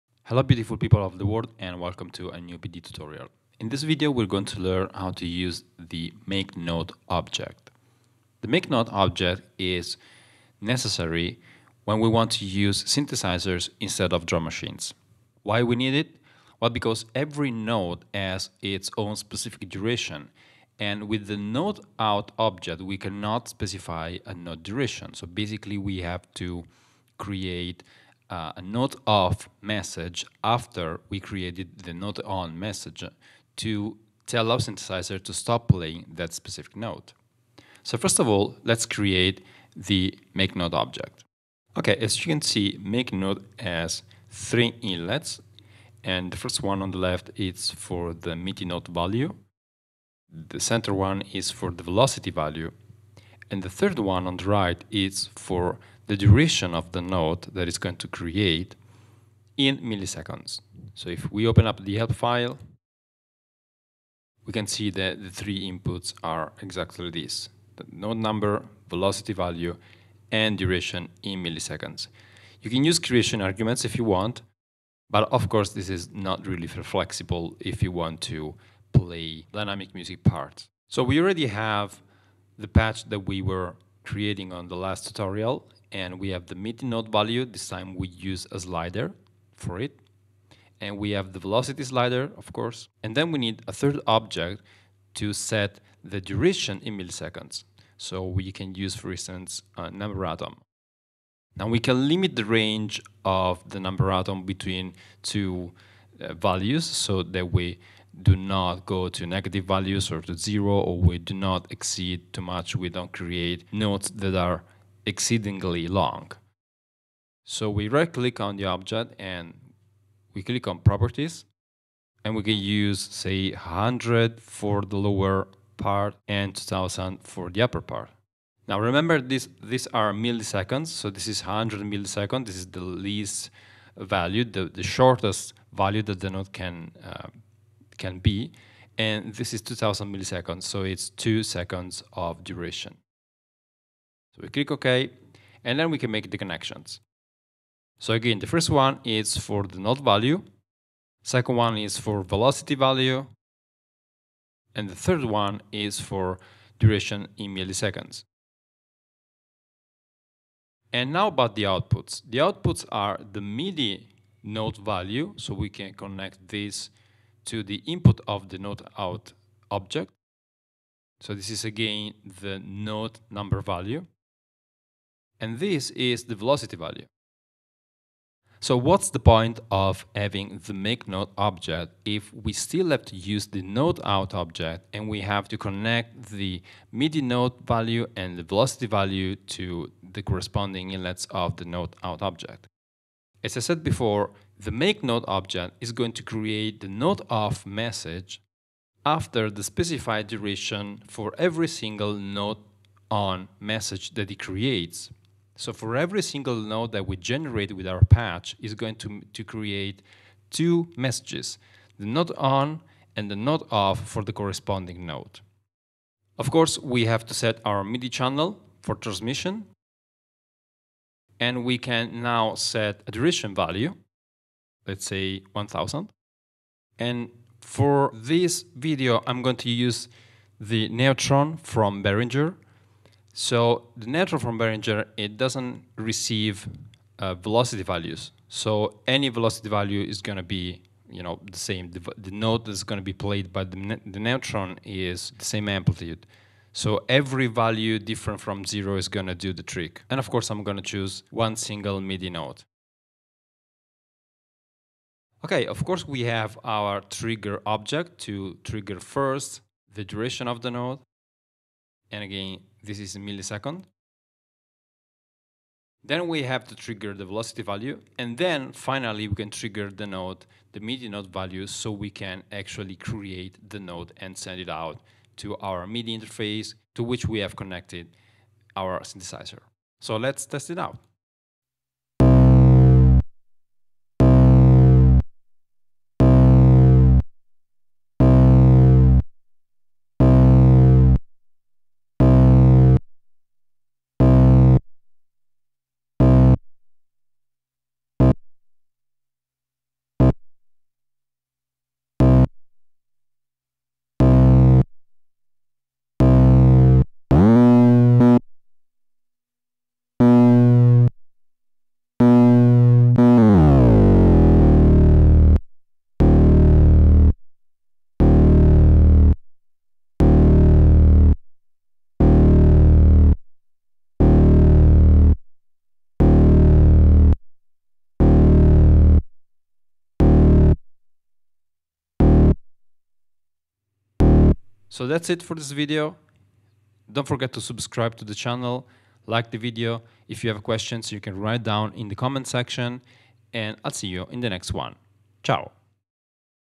Pure Data programming fundamentals Tutorial 10 ENG - MIDI Part 4: controlling hardware synths
In this video we keep learning how to control external synths and drum machines from within Pd patches.

The Muselectron Studio:
My books